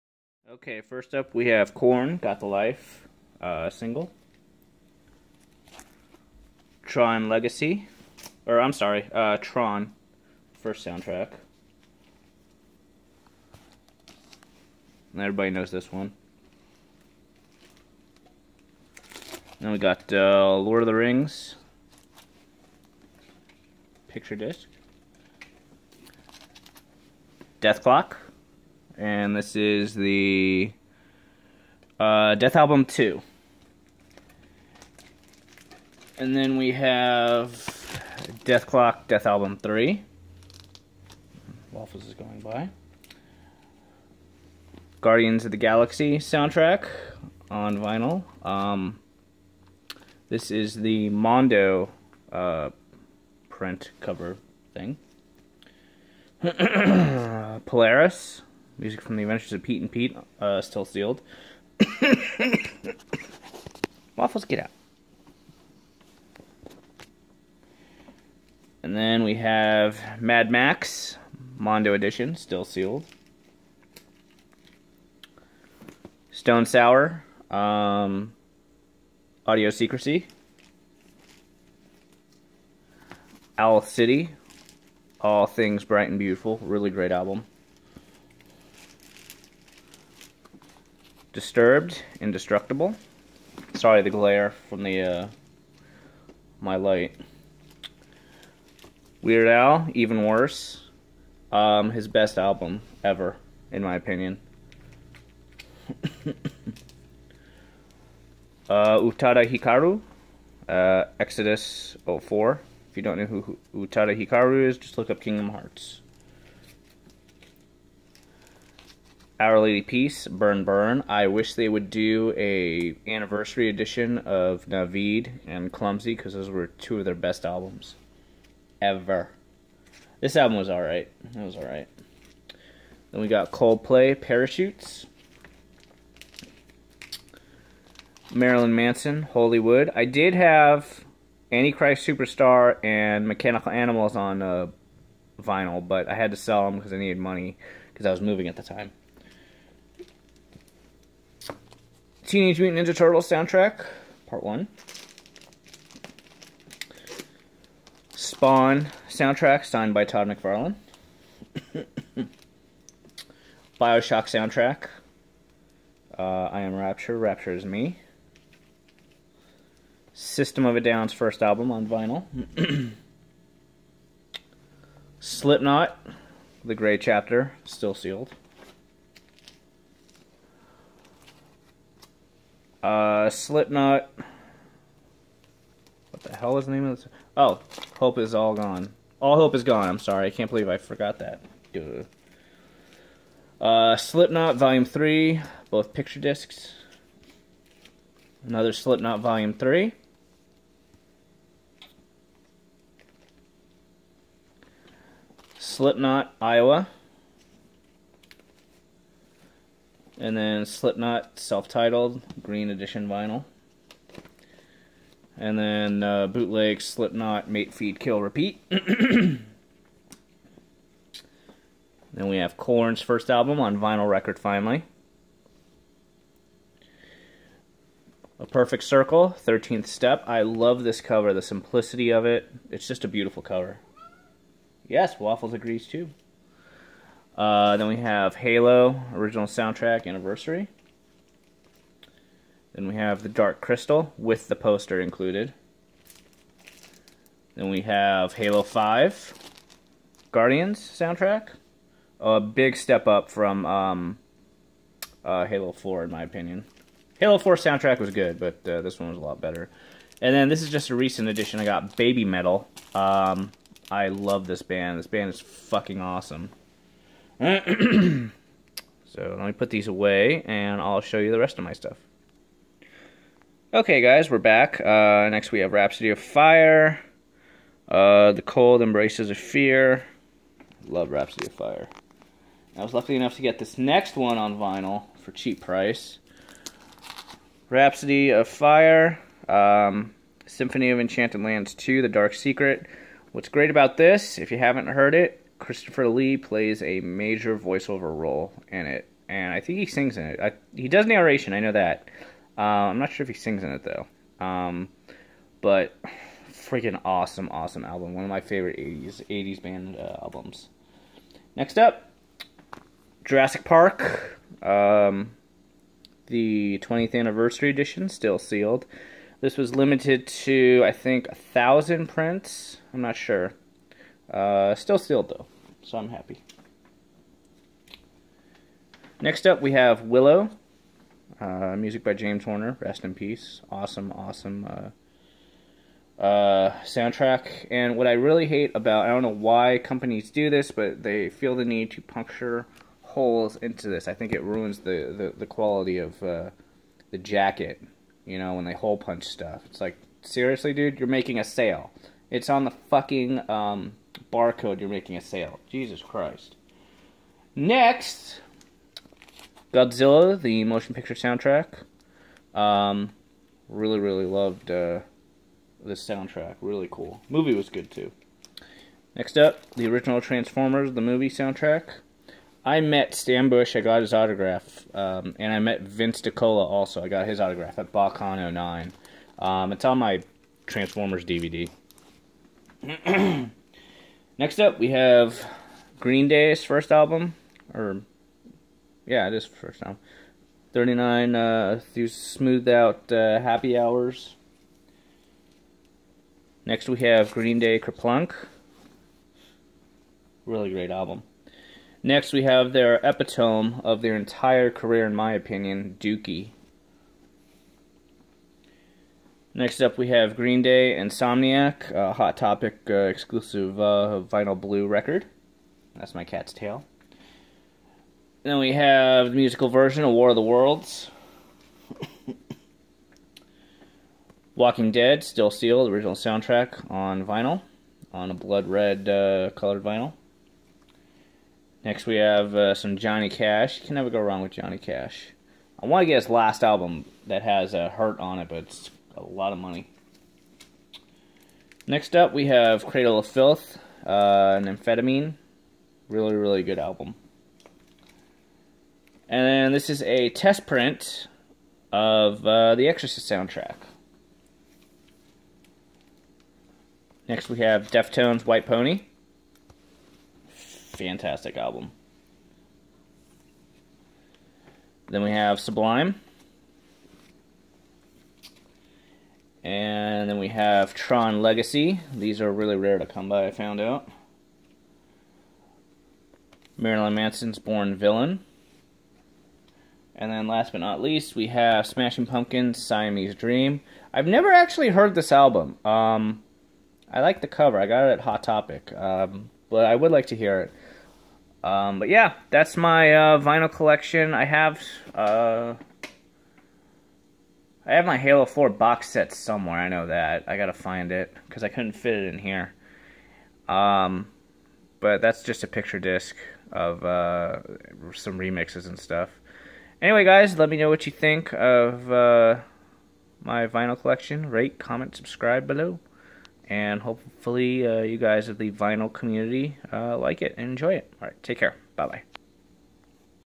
Vinyl LP Collection 2016
Here are my vinyl records of my collection thus far. Enjoy VC!!